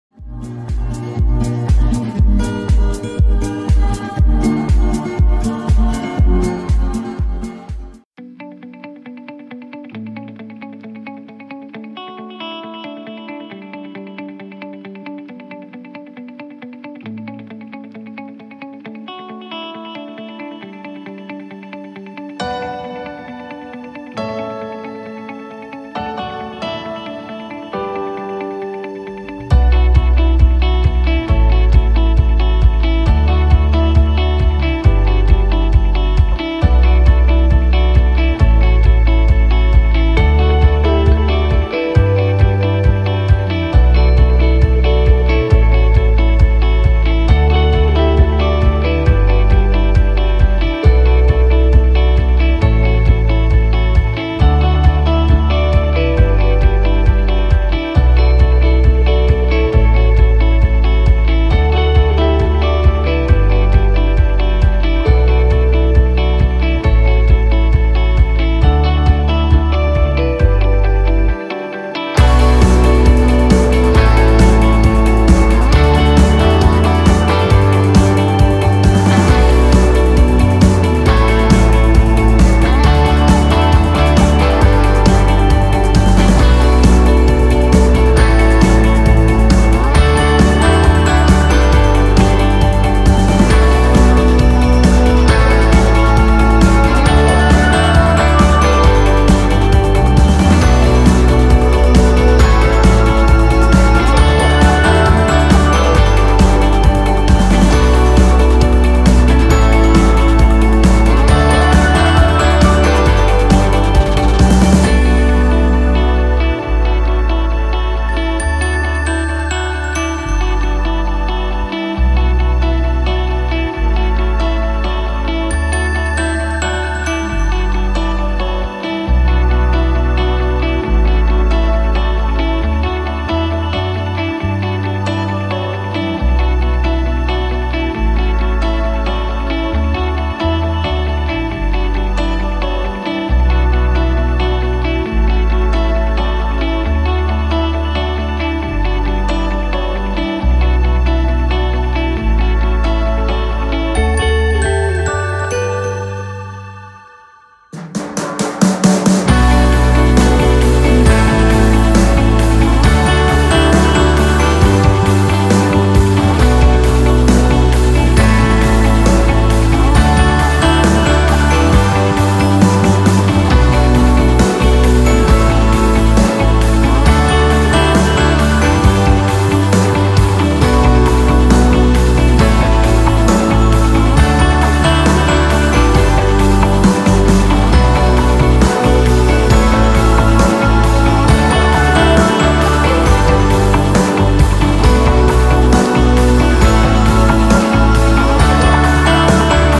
Recycle Plastic Bottles into Bees shaped plant Pot || hanging garden DIY
Transform old plastic bottles into beautiful plant pots. This is  hanging  DIY garden idea. Creative and cute ideas to make at home. Suitable for growing herbs or small plants.